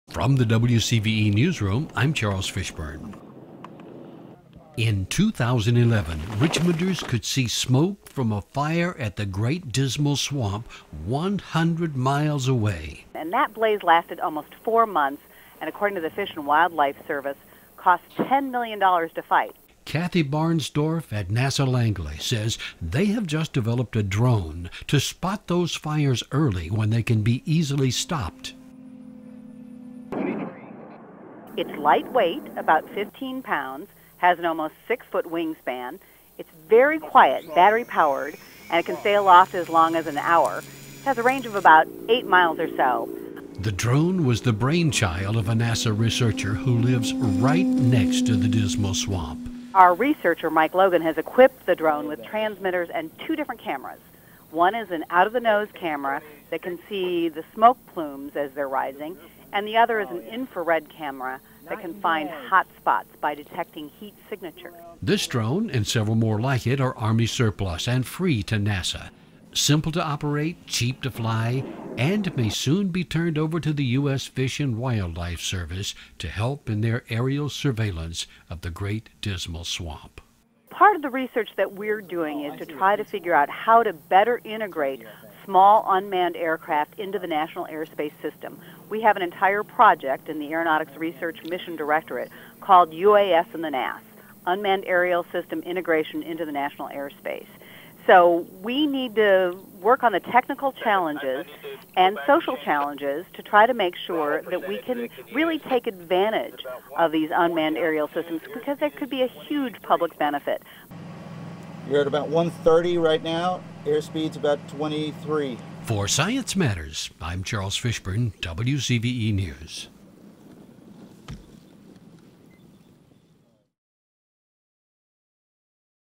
Swamp Drone
NASA DEVELOPING PLAN TO USE DRONES TO HELP SPOT FIRES IN THE DISMAL SWAMP EARLY